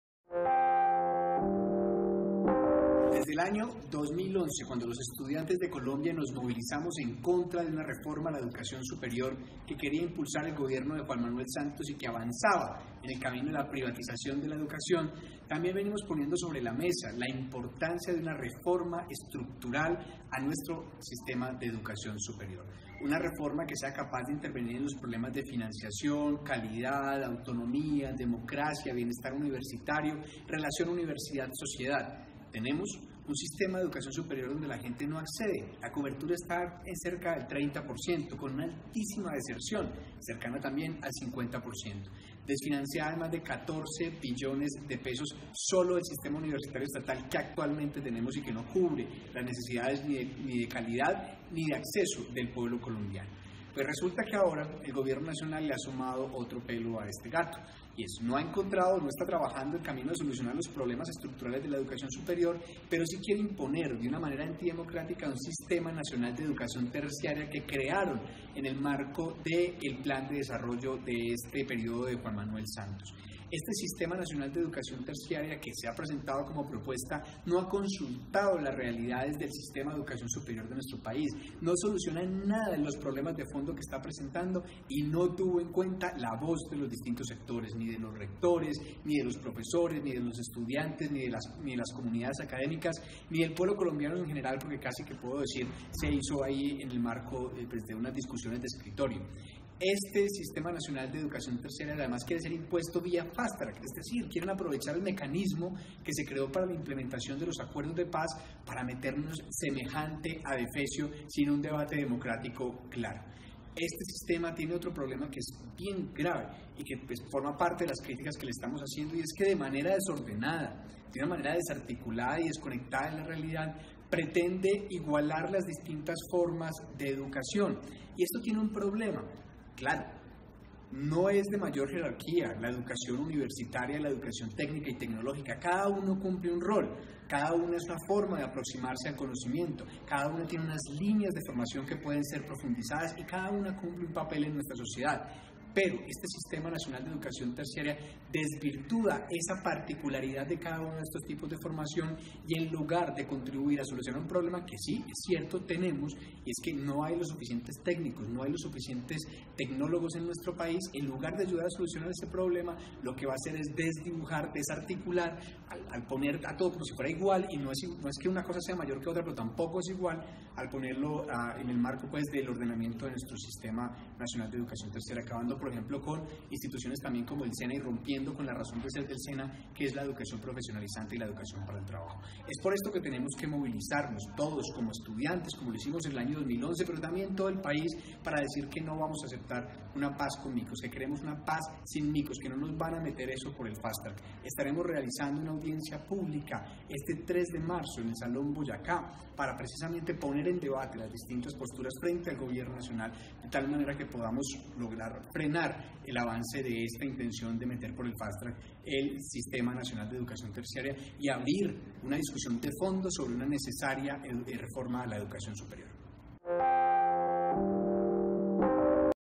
Invitación Audiencia sobre SNET
Invitación del Representante a la Cámara por Antioquia Victor Correa Vélez a la audiencia Pública sobre SNET (Sistema Nacional de Educación Terciaria)